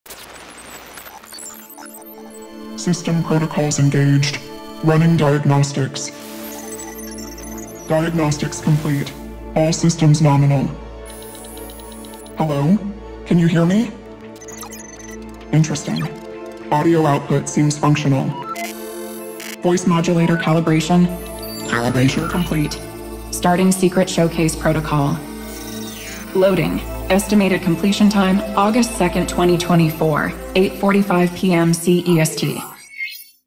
HandyGames AI-Powered Showcase 2024 - Teaser
HandyGames is thrilled to announce the HandyGames Showcase 2024, which will be part of the THQ Nordic Digital Showcase 2024. The event will kick off on August 2nd, 2024, at 8:45 PM CET.

Gaming enthusiasts can tune in to watch the full showcase on the THQ Nordic Showcase Page, YouTube, and Twitch, or view the HandyGames Showcase separately on the official HandyGames YouTube channel. Prepare for a showcase crafted with the precision of an AI-driven algorithm and the creativity of a caffeinated game dev.